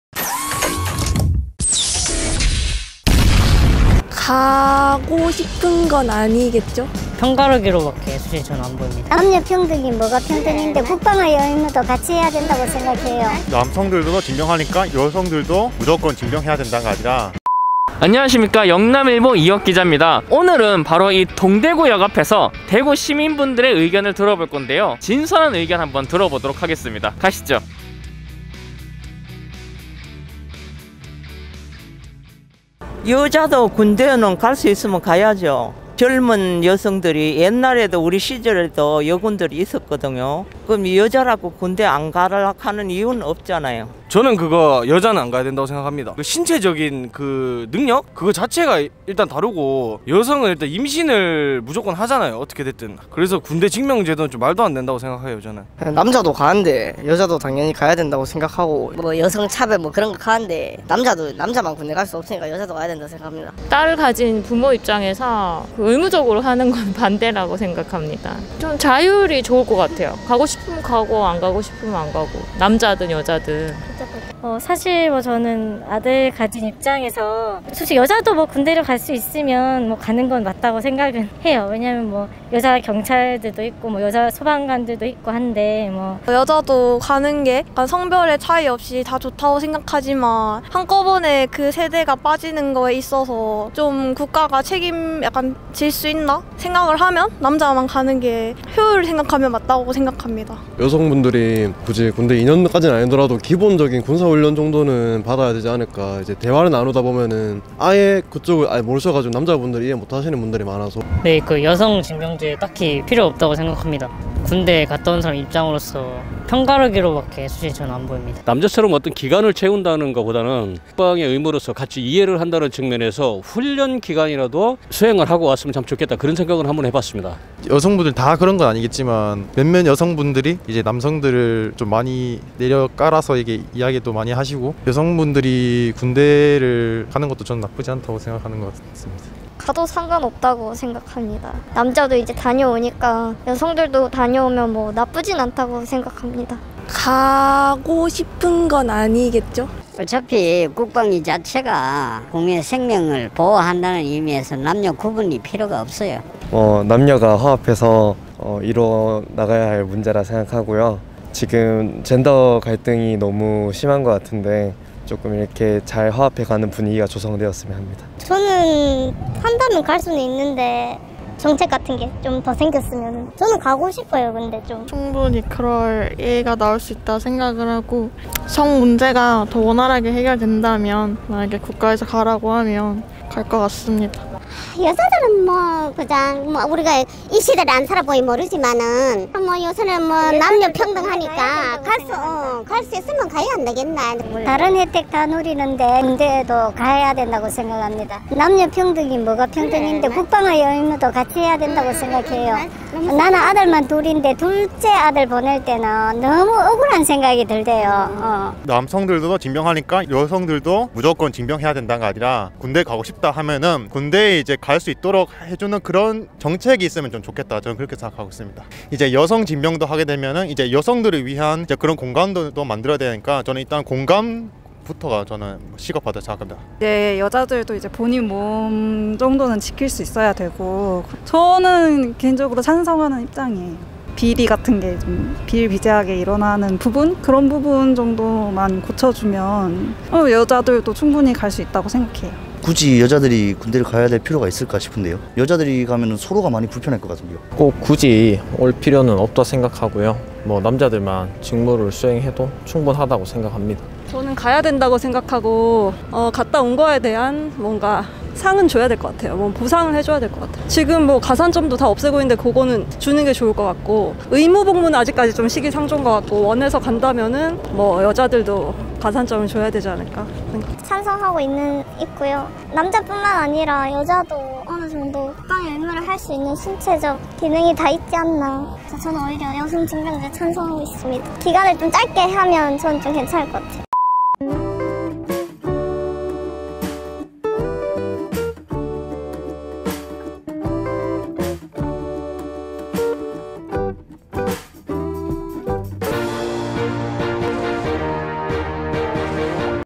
여자도 군대 가라 vs 젠더 갈등 멈춰! [여성징병제 찬반 인터뷰]| 영남일보
대구·경북지역 종합일간지 '영남일보'

대구 시민들은 여성징병제를 어떻게 생각하고 있을까?
동대구역 앞 광장에서 직접 들어봤습니다!